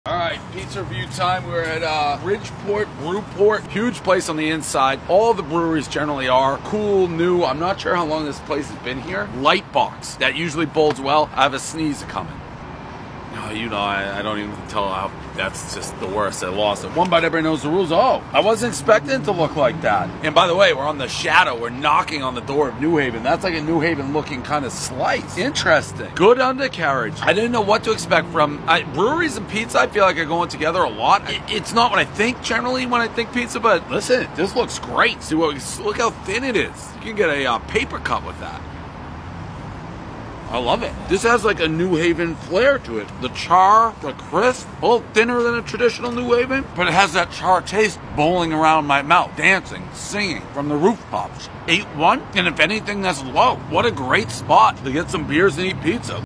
You Can Get A Paper Cut From The Pizza At Brewport
You can get a paper cut from the pizza at Brewport in Bridgeport